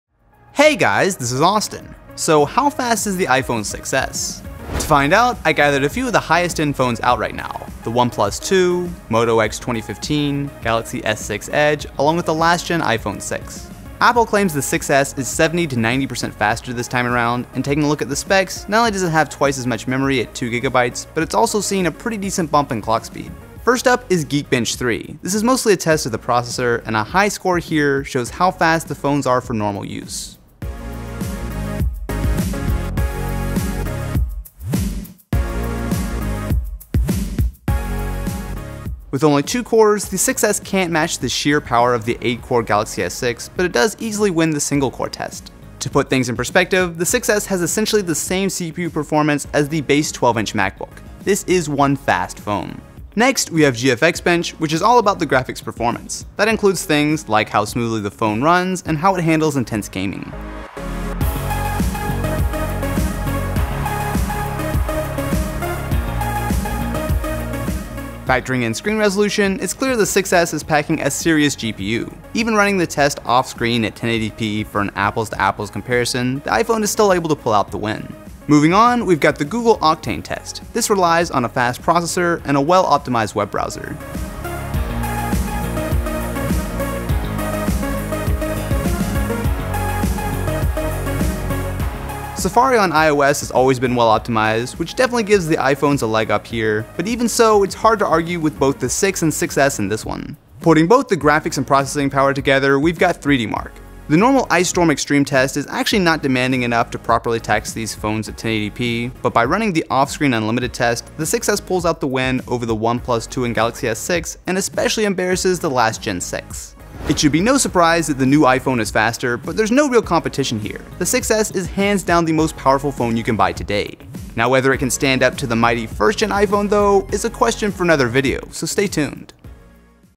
iPhone 6s: How Fast?
iPhone 6s Speed Test vs Galaxy S6, iPhone 6 and more!
With the new Apple iPhone 6s claimed to be up to 90% faster I wanted to put it to the test against the Samsung Galaxy S6 edge, iPhone 6, 2015 Moto X 3rd Gen and OnePlus Two in several benchmarks including Geekbench and 3DMark.